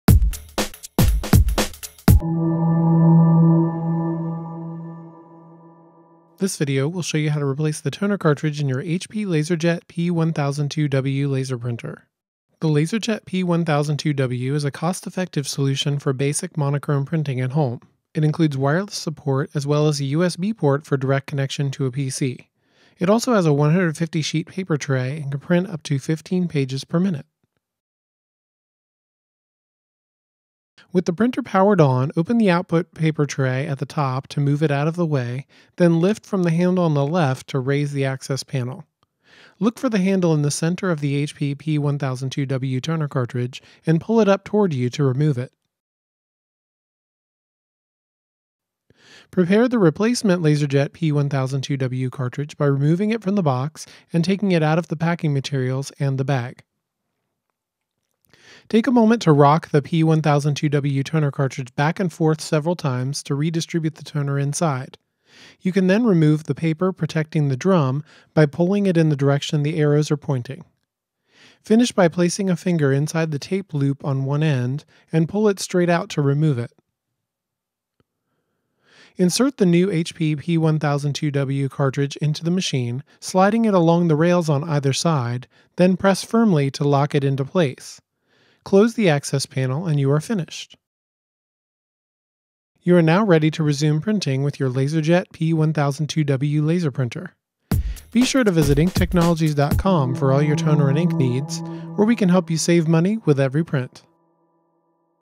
HP LaserJet P1002w Instructional Video
A video demonstrating the replacement of the new compatible HP LaserJet P1002w toner cartridge.